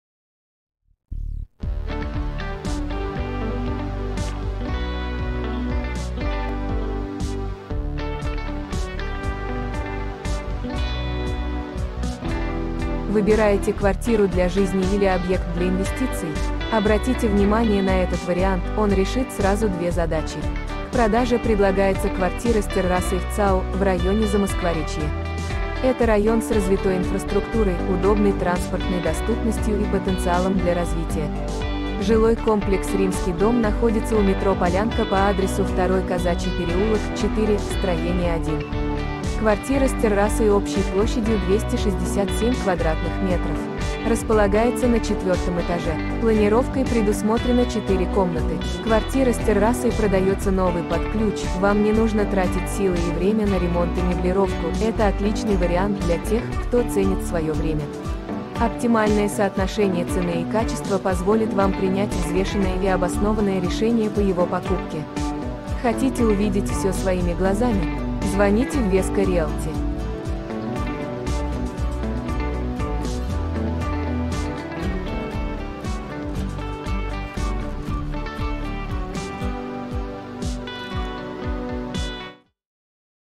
Обзор квартиры: ЦАО, метро Полянка, 2-й Казачий переулок, 4, стр 1 | 61964
Ссылка на объект:
В продажу предлагается пятикомнатная квартира с отделкой в современном стиле площадью 267 кв.м, расположенная на 4 этаже ЖК «Римский Дом».
Планировка: 4 спальни с индивидуальными санузлами и гардеробными, кухня-гостиная, гостевой санузел, кабинет, балкон с колоннами (терраса).
2 машино-места на подземном паркинге.
Окна ориентированы на 3 стороны.